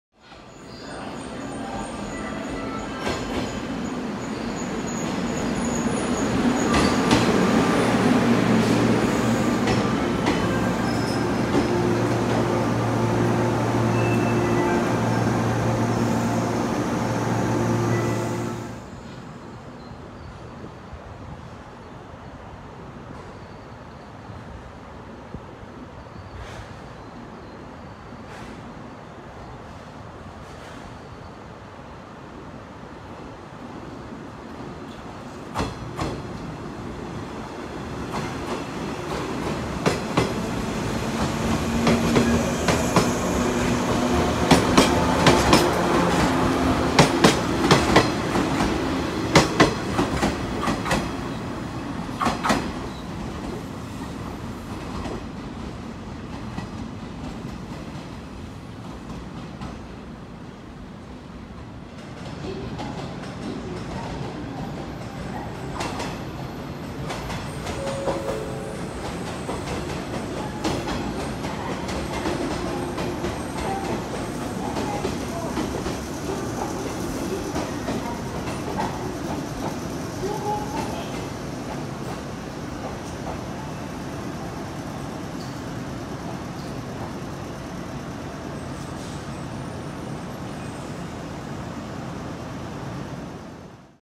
JR西日本 113系濃黄色 P-05編成 可部線運用 広島→あき亀山 2018.7
113系濃黄色 P-05編成です。この日は可部線の運用に入っていました。
2018年7月撮影